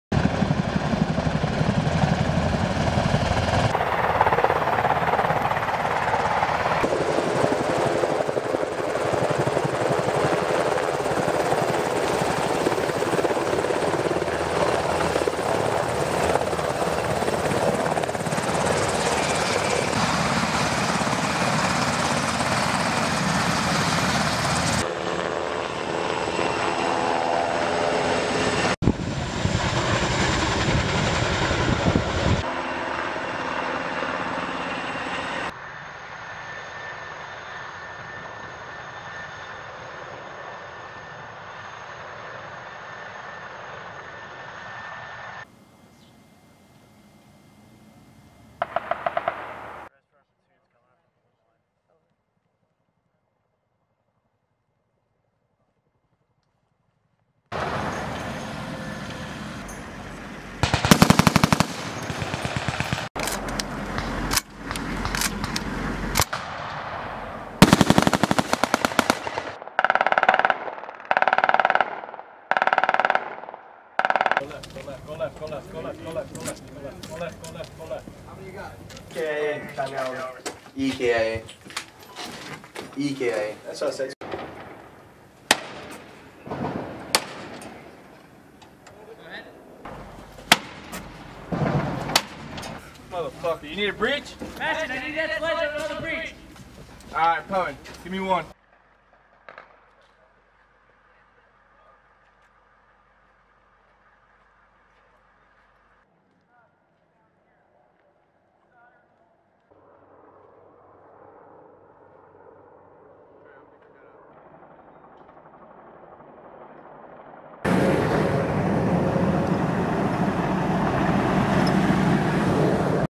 AIR ASSAULT - As seen through a night vision device
With these googles, You can see as far as 200 yards in the dark with only moonlight as a light source.

"Airborne Brigade conduct an air assault operation on a mock town during Operation Anakonda in Wedrzyn, Poland on June 11th."

All the video content used in this clip come from the U.S Department of Defense.